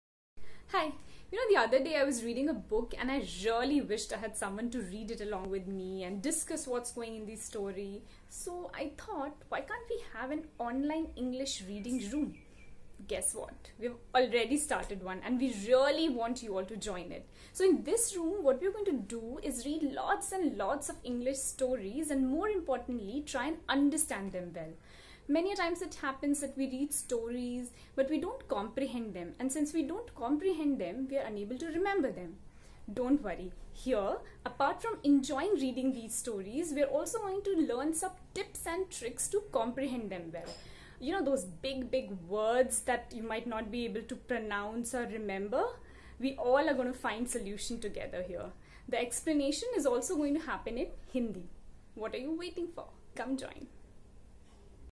READING ROOM - Introduction to the English Reading & Comprehension Course
Join Neha in improving your English Reading and Comprehension. Age Group - 6 to 12 years.